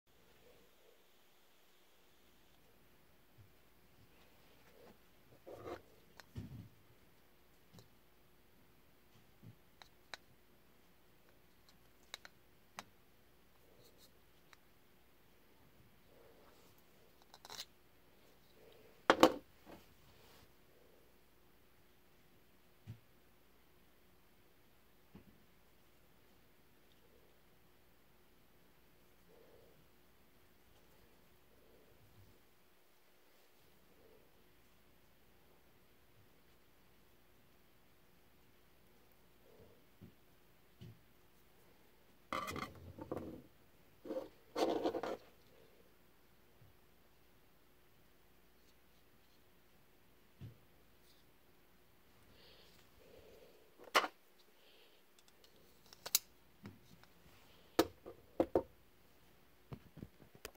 EXTREMA RATIO REQUIEM WOLF GREY BOHLER N690 STAINLESS STILETTO FIXED KNIFE 0478WG
9.25" overall. 4.5" stonewash finish false edge Bohler N690 stainless stiletto style blade. Gray Forprene handle. Gray nylon MOLLE compatible belt sheath. Boxed.

Atlanticknife.com